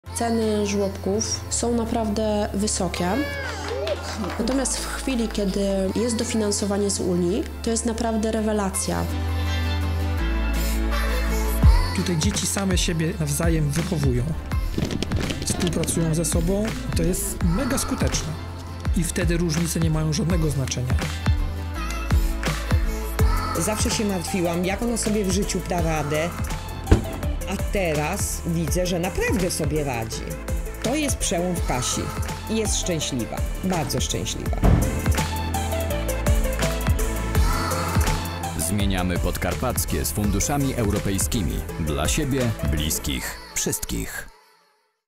Zmieniamy Podkarpackie z Funduszami Europejskimi - Dla siebie. Bliskich. Wszystkich.
Spot jest zwieńczeniem kampanii promocyjno-informacyjnej 2019 pt. Zmieniamy Podkarpackie z Funduszami Europejskimi - Dla siebie. Bliskch. Wszystkich. Przedstawiamy 3 wspaniałe historie naszych bohaterów: Katarzyny Dziedzic opowiadającej o korzyściach bycia uczestnikiem w Stowarzyszeniu Rodziców i Przyjaciół Osób Niepełnosprawnych „Radość w Dębicy” , Karoliny Bieńko wraz z dziećmi korzystająca z usług Żłobka „Zielony Motylek” Jasionka oraz Mariusza Bezdzietnego opowiadającego o Podkarpackiej Sieci Wsparcia Wychowawczej Funkcji Rodzin.